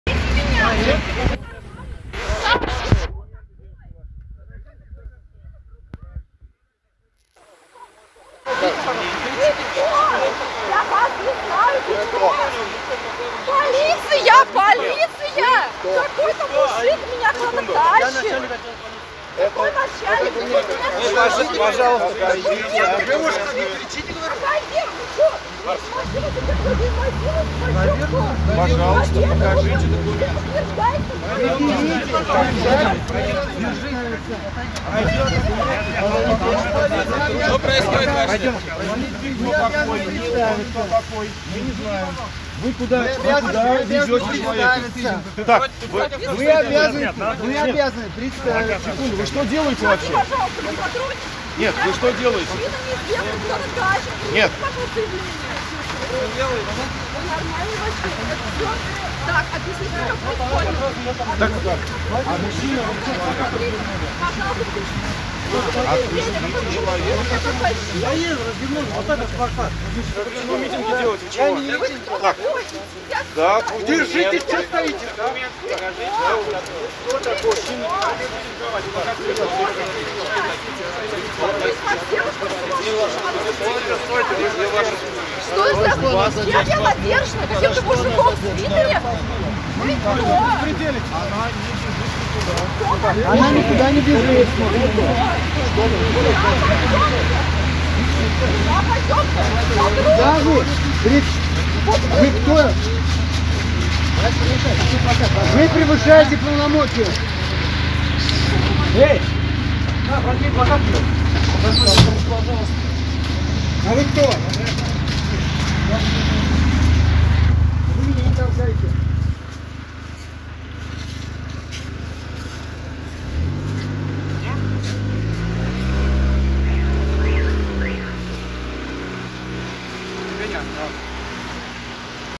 Как полиция задержала экоактивистов. Шиес 2 (Казань)
Посмотрите, как начальник отдела полиции вёл себя с экоактивисткой Верой Керпель.